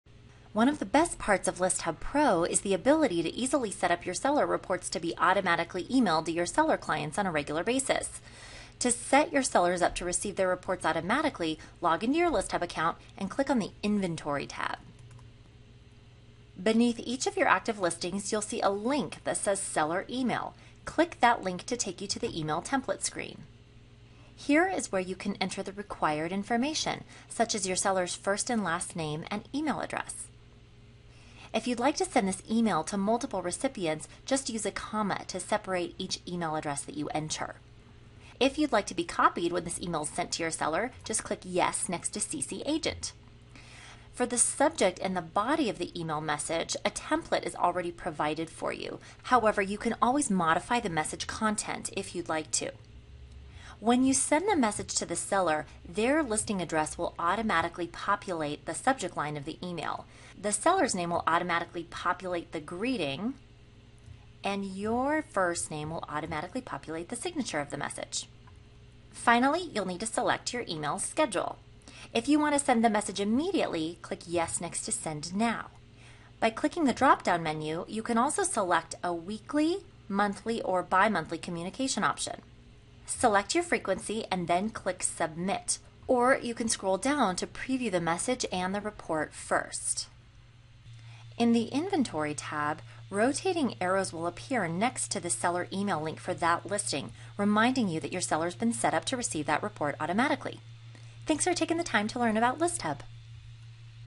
ListHub Pro: Sending Seller Reports to Clients
ListHub Pro customers can learn how to set up Seller Reports to be sent to clients, including how to set up these reports to be sent automatically on a recurring basis.